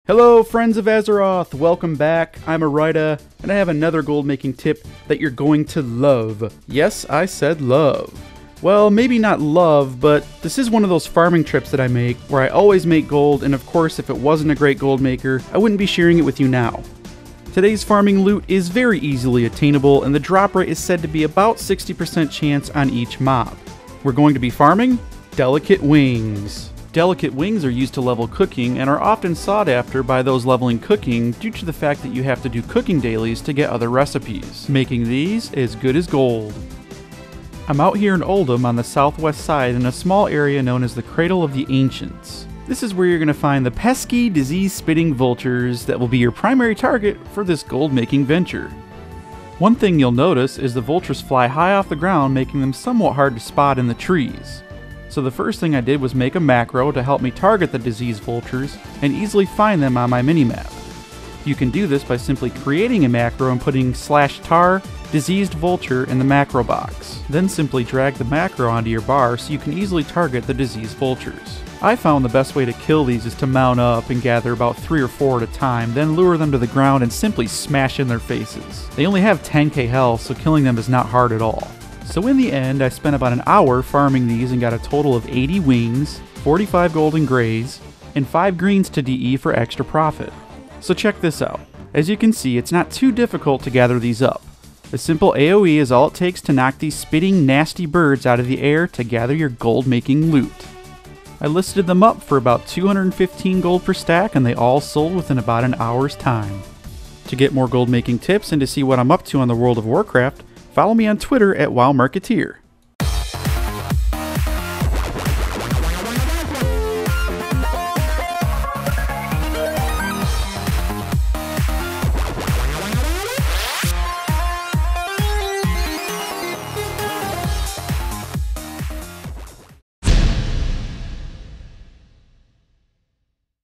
Farming: Delicate Wings
Delicate Wings are used to level cooking and I hardly ever see any on the auction house.  Whenever I farm these I never have problem selling them within a day.  It's an item that's easy to farm and sells fast.  What more could you ask for?  Watch how I do it after the jump!